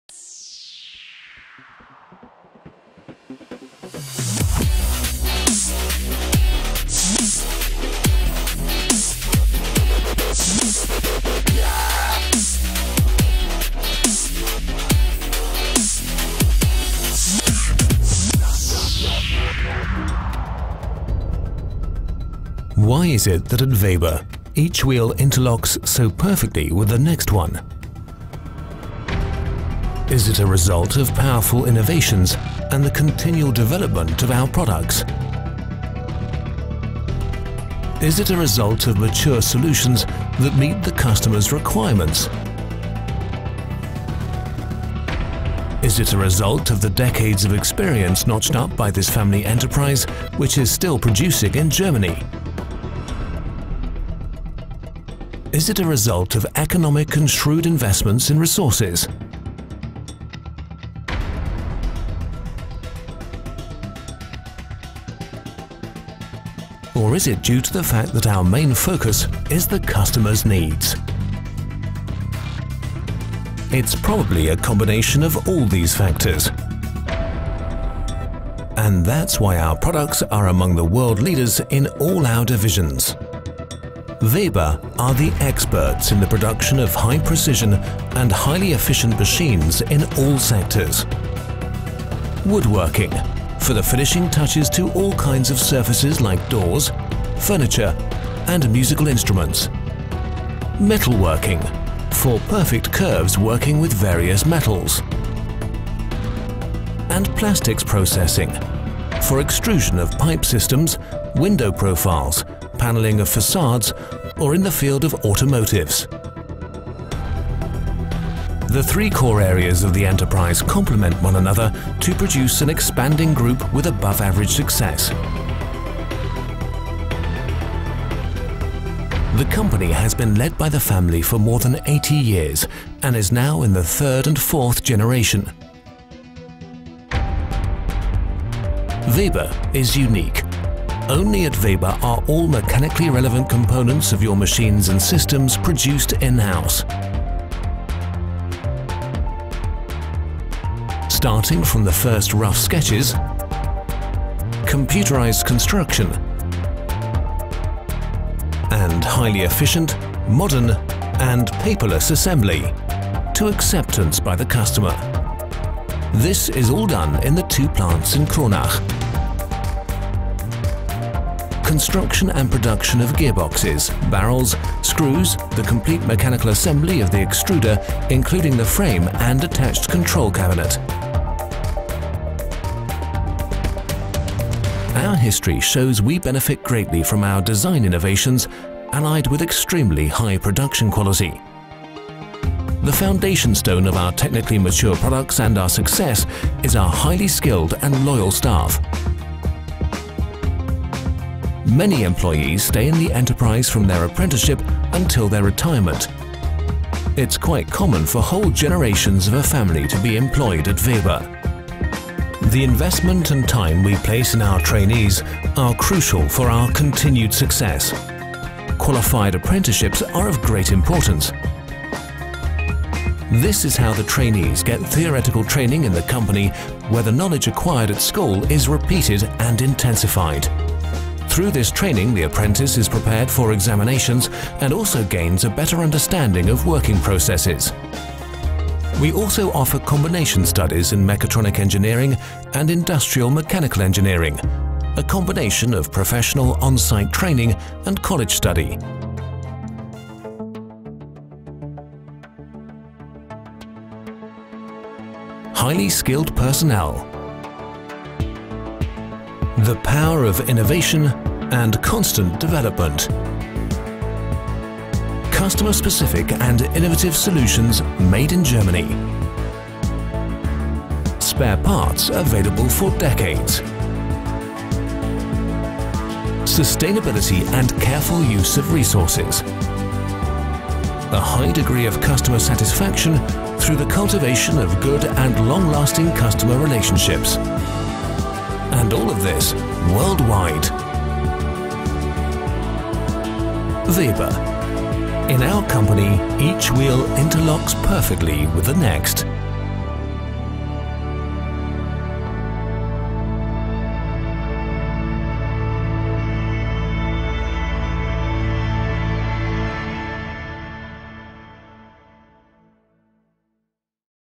WEBER Corporate Overview
Already in the fourth generation, WEBER develops and manufactures machine solutions for the wood and metal finishing industries. Today more than 300 employees, in administration and production, work on the innovative and high-quality machine technologies in the parent company's facility as well as the modern production and development center in Kronach. The company's own pilot plant is constantly testing new processes and developing its own patents. The reward for this...WEBER machines are technically the world leader in all their fields of application.